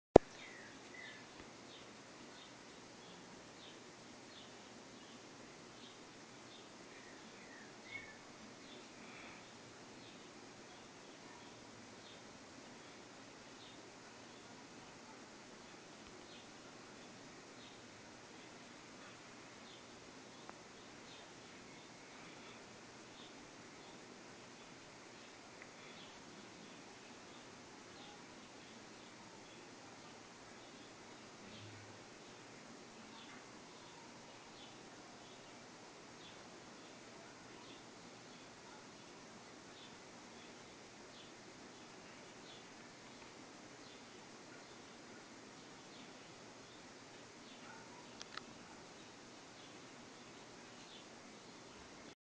DOSBox running Windows95 on BlackBerry PlayBook
Got both DOSBox and Win95 running on the PlayBook. It was fairly easy and thing is running quite fine. Once you get used to the gui/commands... Quite interesting.